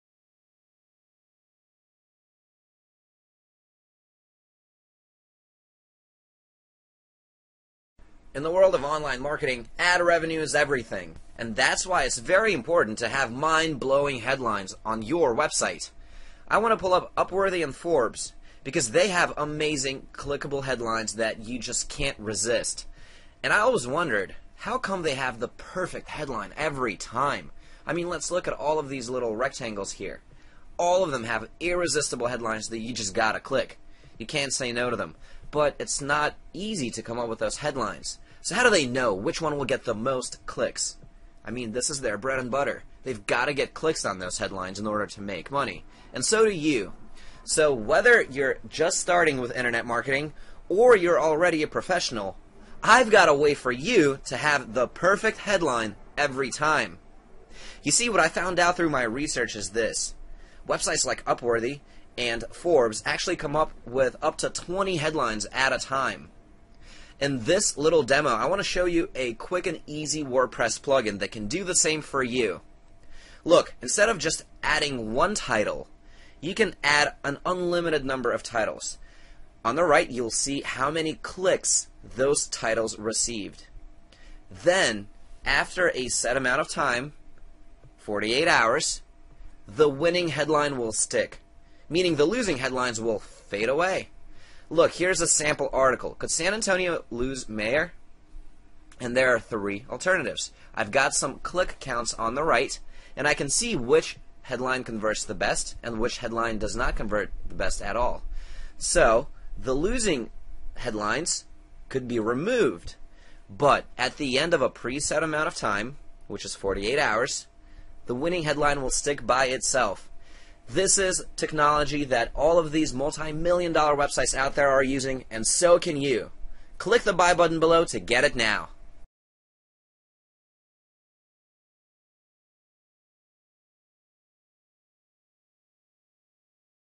WP Titles Plugin | WP-Titles-Demo-Review
WP Titles Plugin, This Stupid - Simple Plugin For WP Lets You Find The Best Converting Title For Each Of Your Posts, On Autopilot, Guaranteed!

WP Titles Plugin will show you why you've been missing out on optimal clicks your whole life,
and it will show you how to fix this problem. The more clicks you get, the more money you can make. Thanks to this simple formula, people just like you are making full-time incomes.
But even if your site is already established, you can boost your conversions by installing WP Titles today.

- It boosts clicks on all of your new & existing posts
- It automatically chooses winning title for each page
- It works with all WP sites

And With WP Titles Plugin If your headline fails to seduce the reader, it will not get a click. Thanks to this simple plugin, you can now have certainty that was never available to anyone, anywhere.  It's the certainty of always having the best title - which is arguably the most important part of any page!